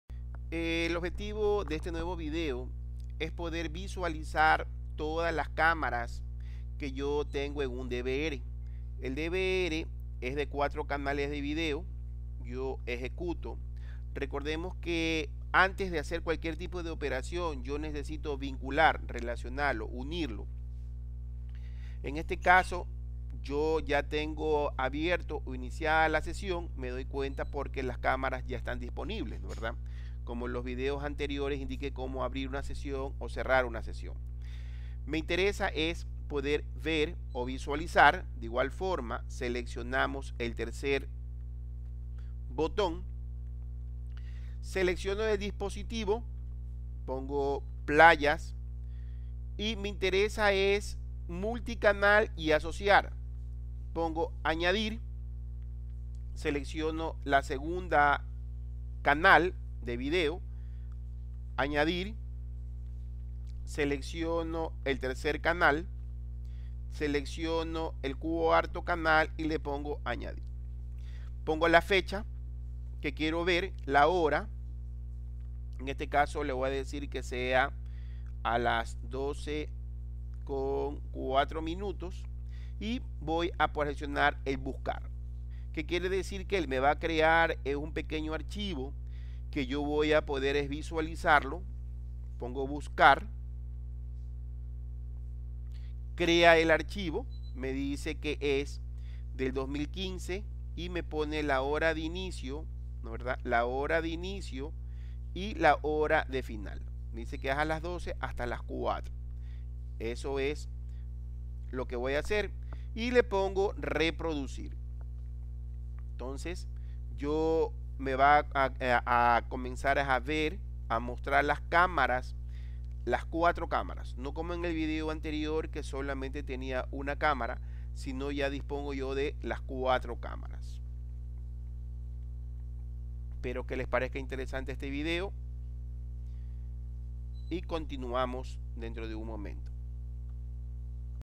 05 PSS DAHUA - Ver lo Grabado todas las camaras
Con el software pss de dahua, como puedes visualizar el historial de las cámaras.

cámaras de seguridad, Bits Cia Ltda,  vendemos, instalamos y configuramos en su celular, table etc, en todo el ecuador, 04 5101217